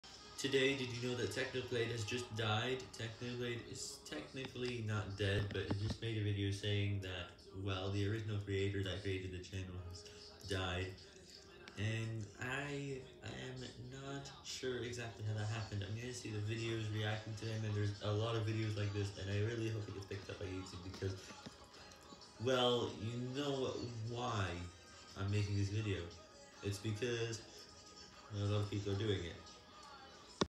has died…
The original creator who made @Technoblade had died and some people are talking about it. I have added some end screens going to this video.